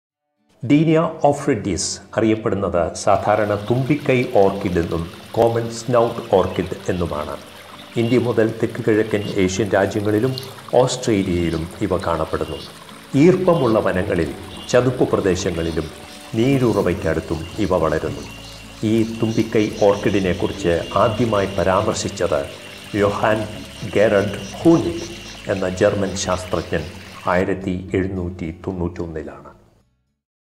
Common Snout Orchid (ഡീനിയ ഒഫ്രിഡിസ്)
Dienia ophrydis, commonly known as the Common Snout Orchid is a plant in the orchid family and is native to endemic to a broad area of Asia, Southeast Asia, the Philippines, New Guinea and northern Australia. It is a deciduous, terrestrial orchid with a cone-shaped stem, bright green, wavy leaves and many greenish, brown, reddish or purplish flowers crowded on a wiry flowering stem.